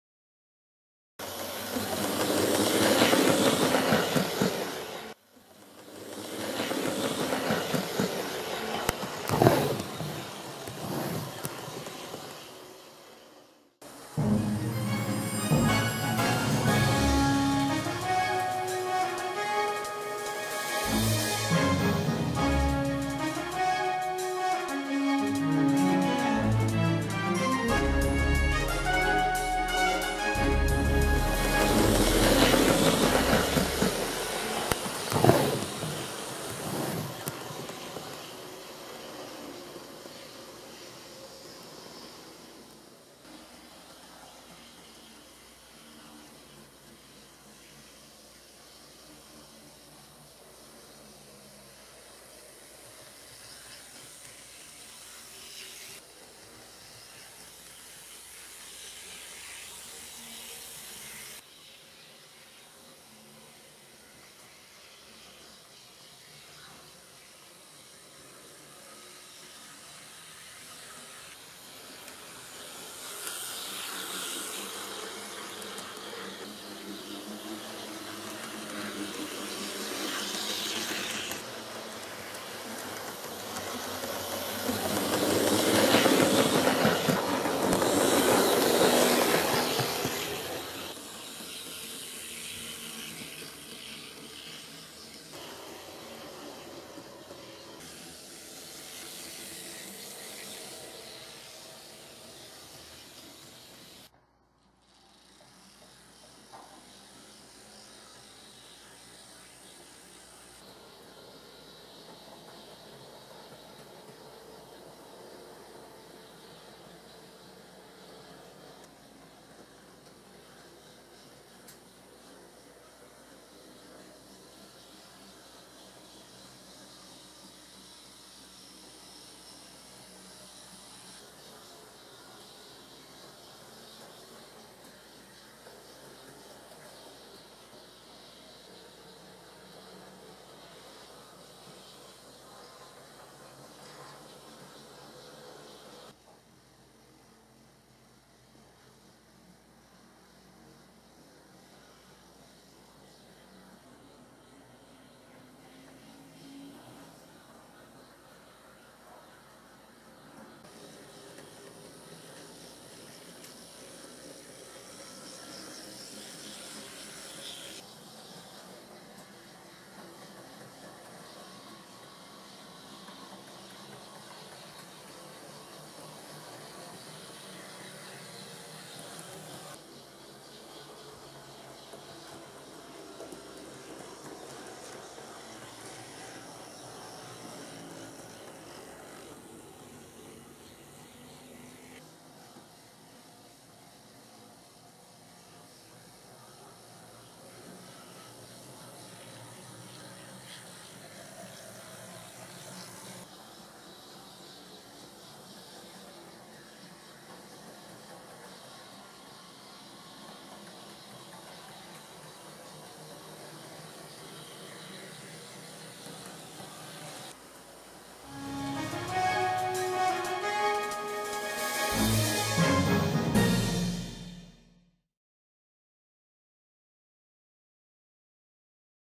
EH10による試運転
第1期工事が完成。線路の状態を確認するため最も重たいEH10を、外回りの「大分の鉄道物語線」、内回りの「竹田ー岡城/荒城の月線」を走らせました。